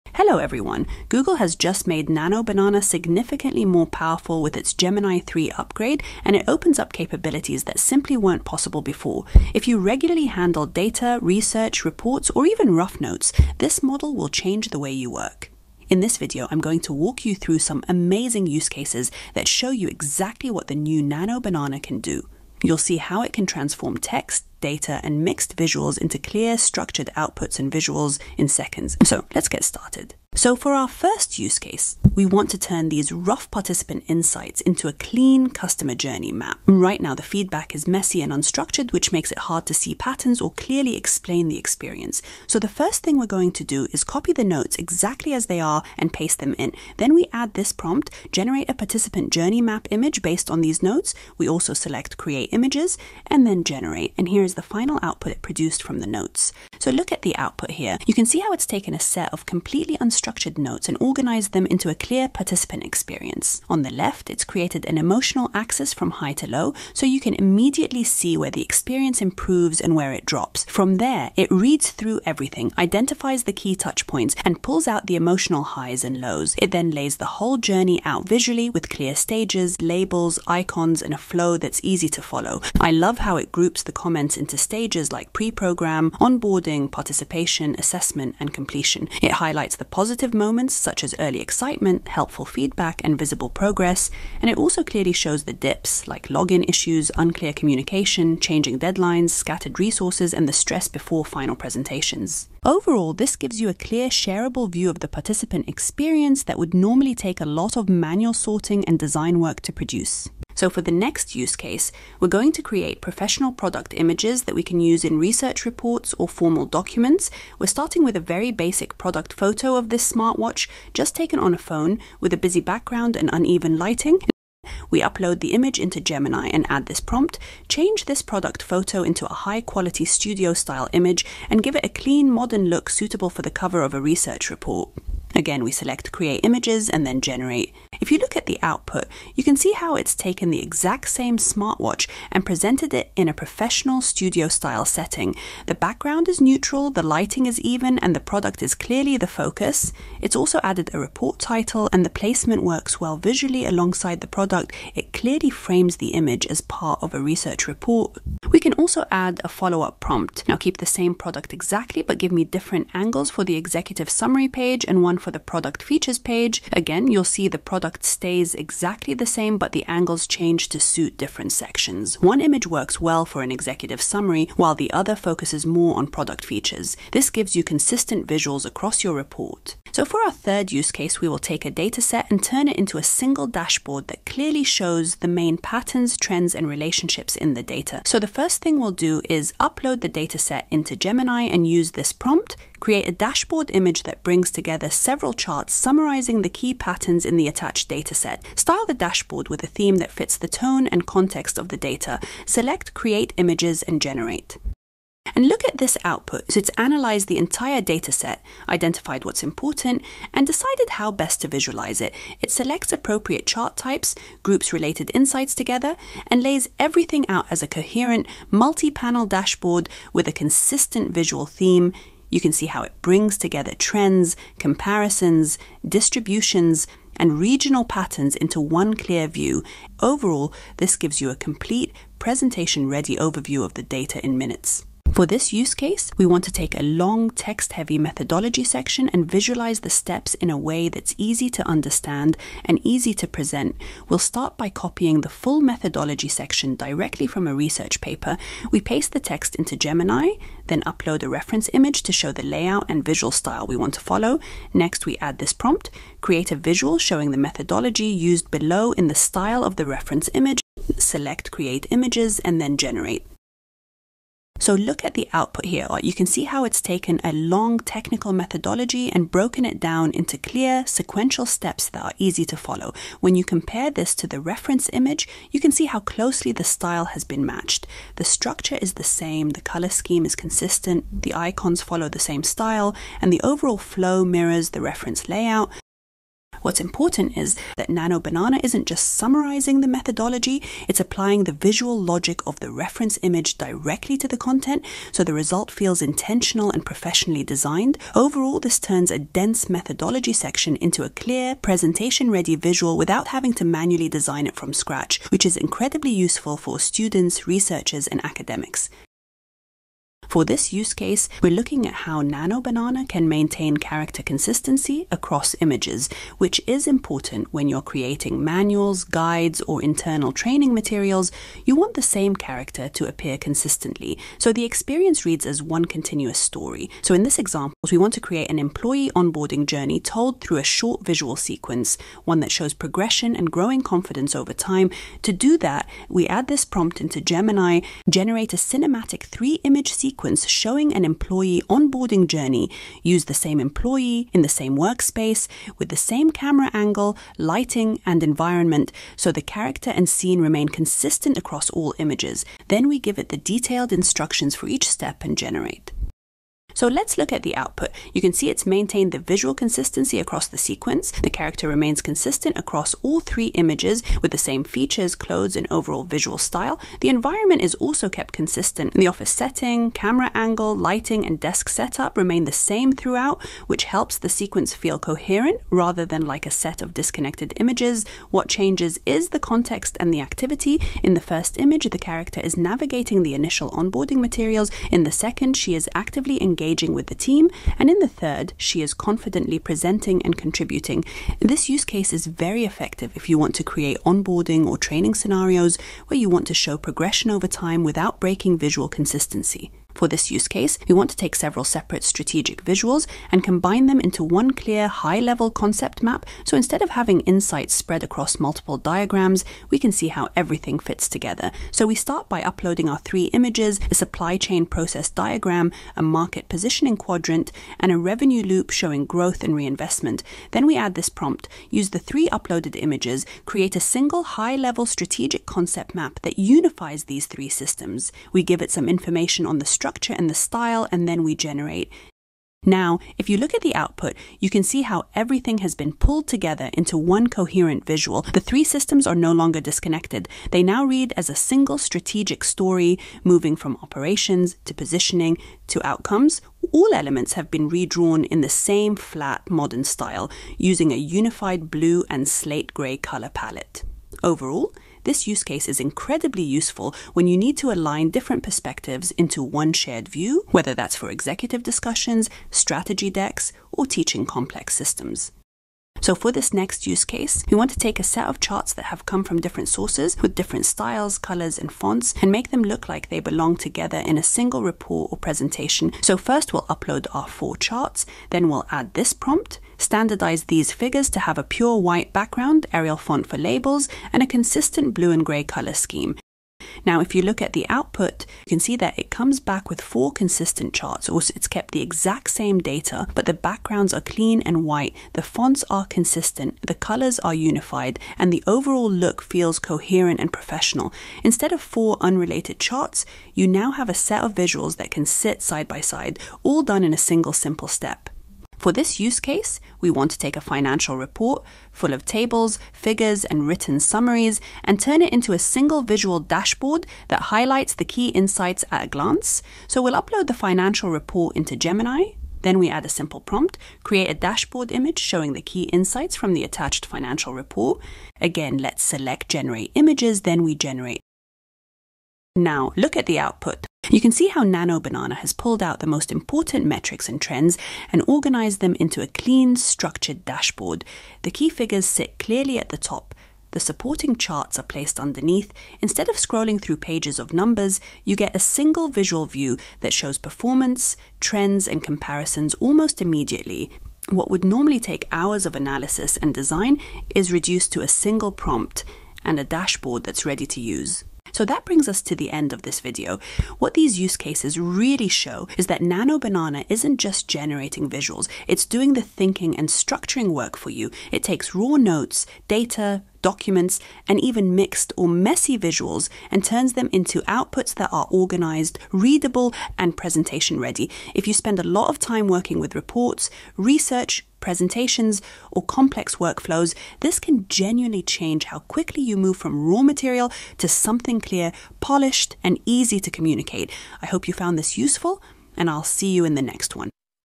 Insane Nano Banana Pro Use Cases That Help You Save Hours!
In this video, I walk through a number of Nano Banana Pro use cases that show how the new Gemini 3 upgrade changes the way visuals are created.

You’ll see how Nano Banana Pro goes beyond basic image generation and starts handling the structuring and organisation work for you. Instead of manually cleaning data, sorting notes, or redesigning visuals, the model takes raw inputs and turns them into clear, presentation-ready outputs in seconds.

The examples in this video focus on real workflows, working with messy notes, datasets, documents, mixed visuals, and onboarding scenarios, and show how Nano Banana Pro can support research, reporting, presentations, and everyday professional work.